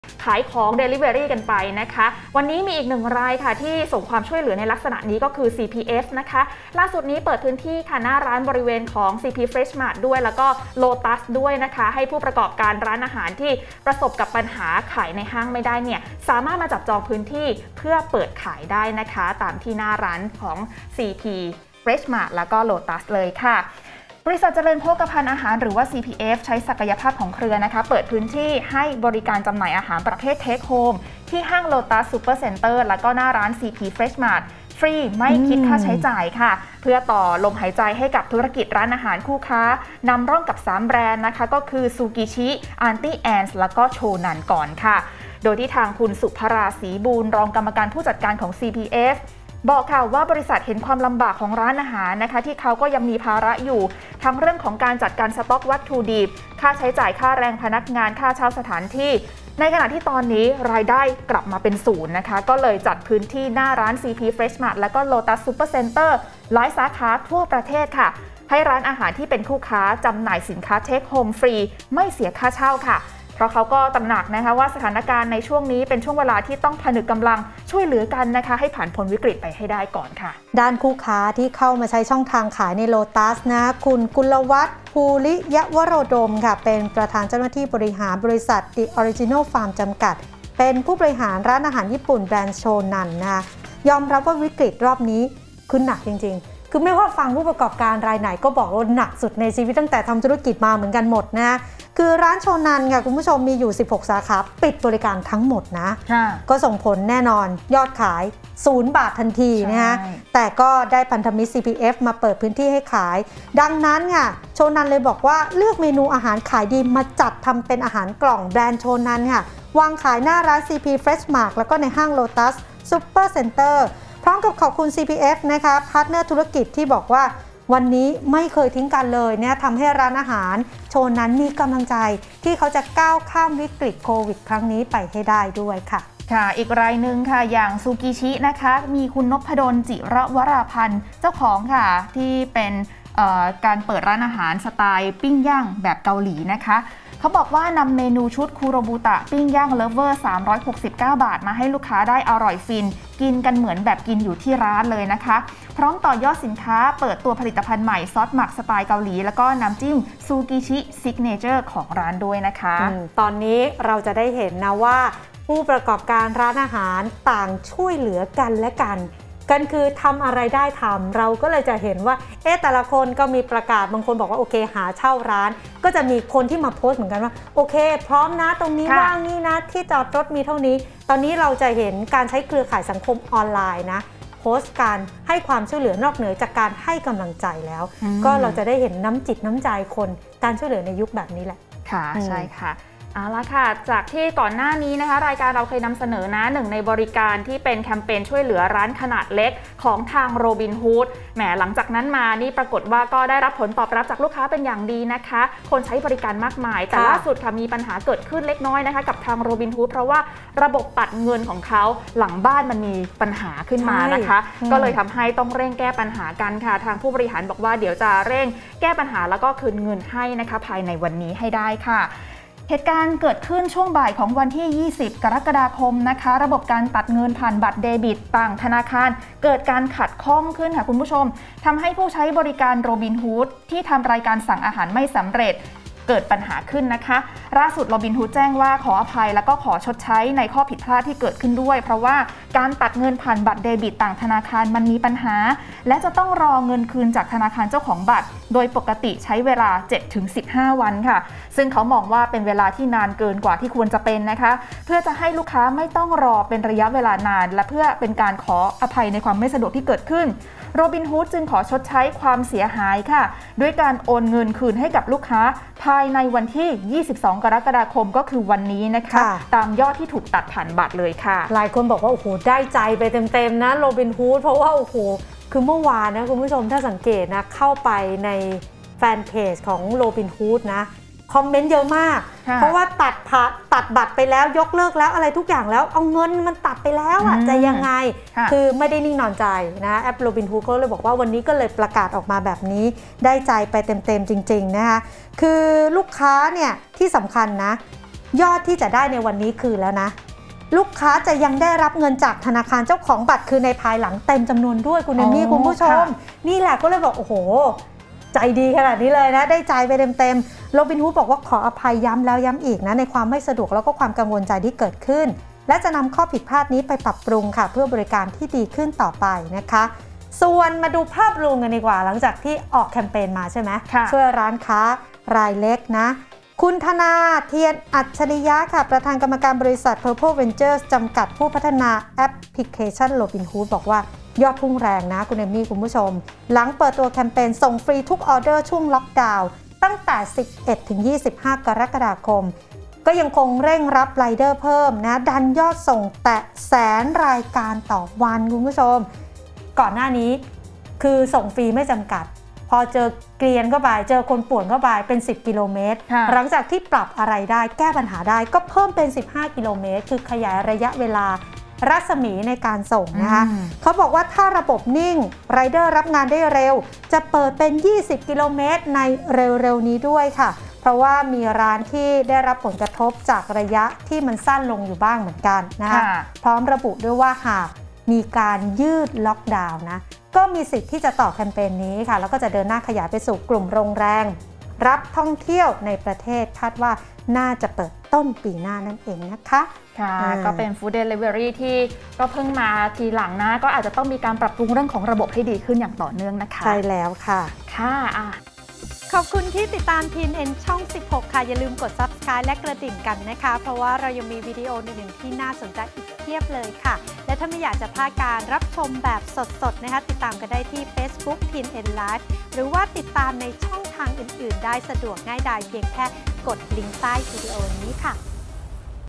"โรบินฮู้ด" เร่งคืนเงินลูกค้าวันนี้หลังระบบมีปัญหา I การตลาดเงินล้าน I 22 07 64
"โรบินฮู้ด" เร่งคืนเงินลูกค้าวันนี้หลังระบบมีปัญหา I การตลาดเงินล้าน
โรบินฮู้ด ชี้แจงหลังระบบตัดเงินบัตรเดบิตต่างธนาคารมีปัญหา สร้างความเสียหายให้ลูกค้า เตรียมคืนเงินตามจำนวนให้ลูกค้าวันนี้ (22 ก.ค.64)